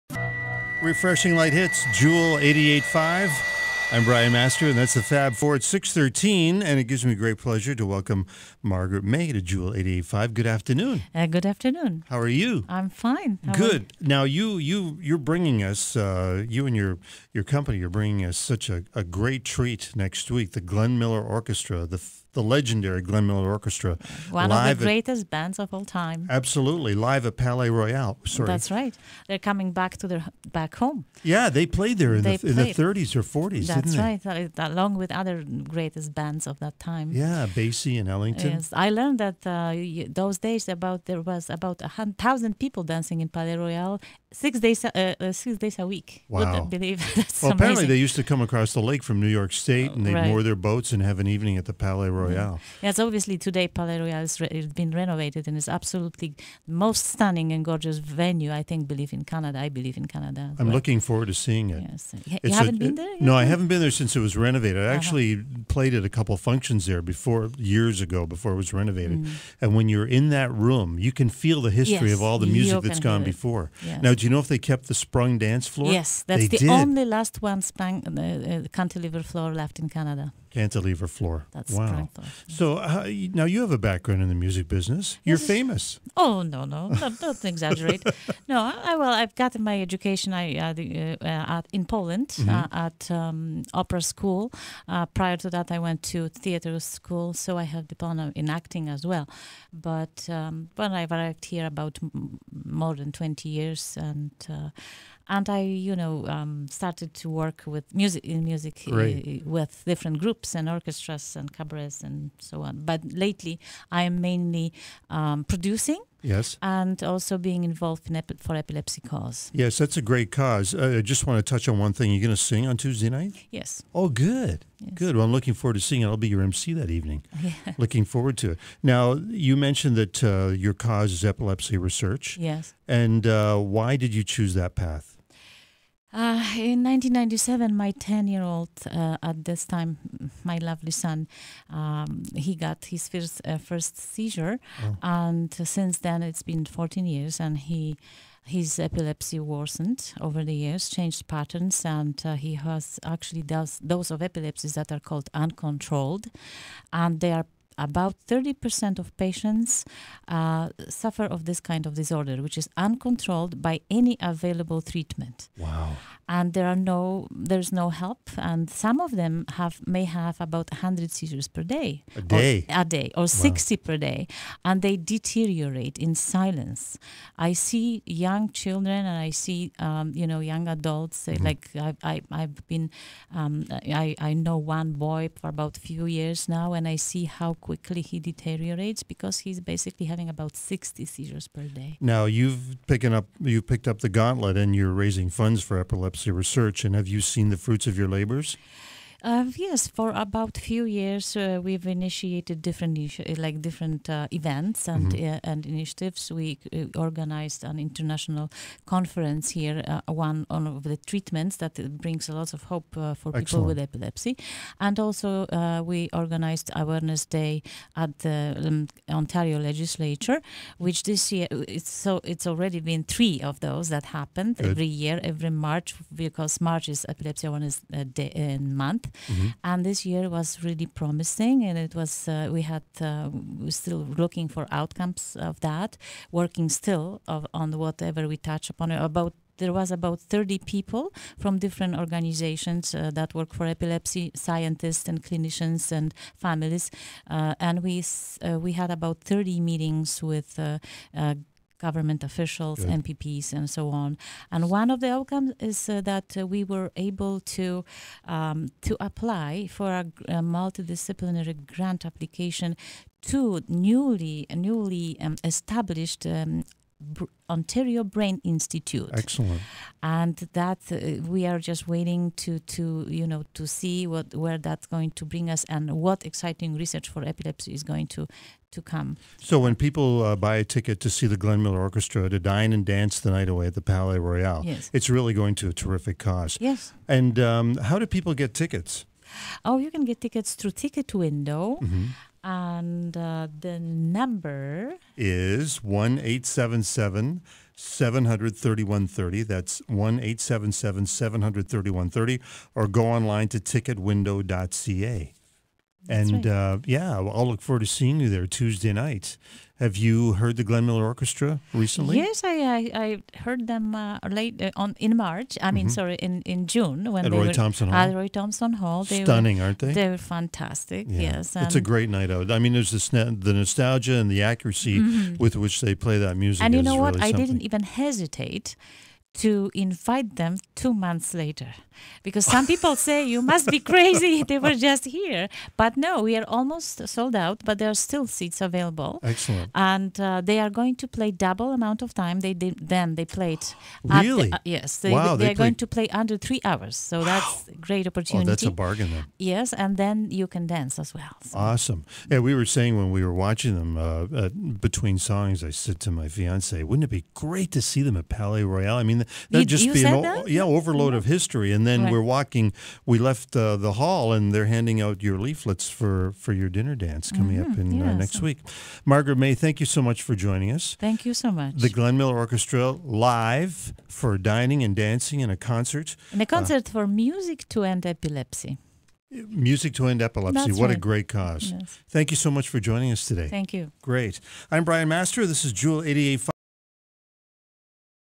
Margaret Maye Jewel 88.5 interview
Interview on Jewel 88.5 with Margaret Maye and Brian Master Program Director, Jewel Radio Network. Margaret speaks about the upcoming benefit concert for the University of Toronto Epilepsy research program with the world famous Glenn Miller Orchestra under the direction of Gary Tole at the Palais Royale located in the riding of Parkdale - High Park in Toronto which is represented by MP Peggy Nash and MPP Cheri DiNovo.